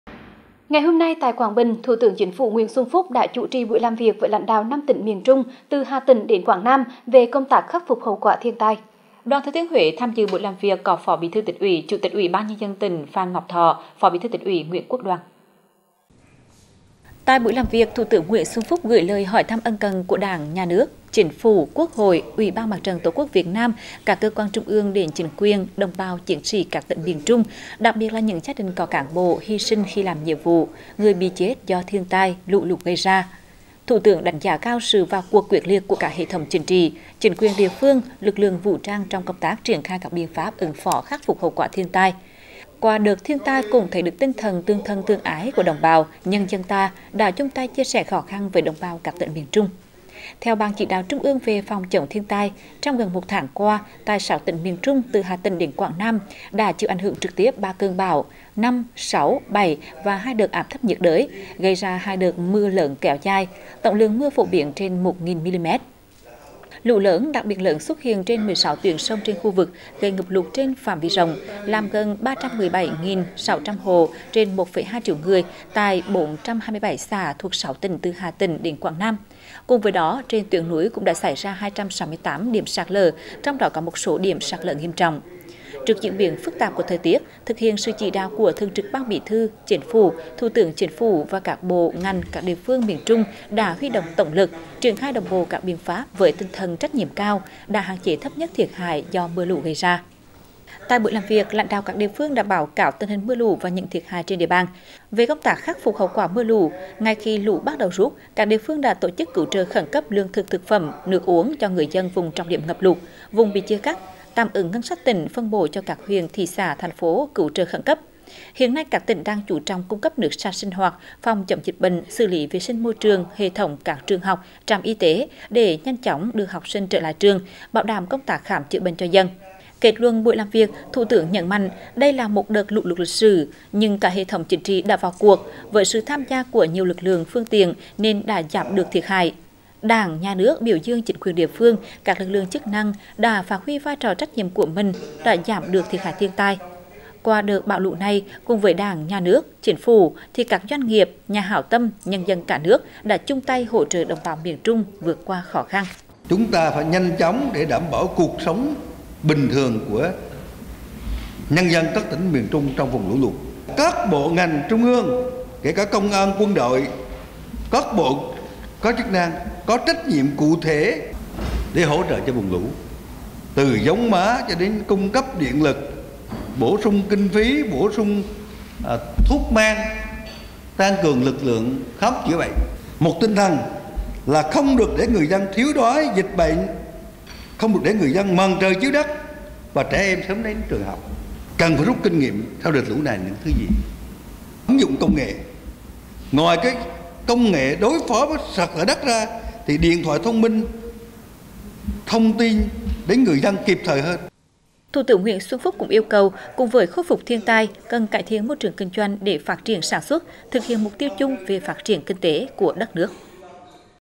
Thủ tướng Chính phủ làm việc với lãnh đạo các tỉnh miền Trung về khắc phục hậu quả thiên tai
Ngày hôm nay, tại Quảng Bình, Thủ tướng Chính phủ Nguyễn Xuân Phúc đã chủ trì buổi làm việc với lãnh đạo 5 tỉnh miền Trung từ Hà Tĩnh đến Quảng Nam về công tác khắc phục hậu quả thiên tai.Đoàn TT Huế tham dự buổi làm việc có Phó Bí thư Tỉnh ủy, Chủ tịch UBND tỉnh Phan Ngọc Thọ; Phó Bí thư Tỉnh ủy Nguyễn Quốc Đoàn.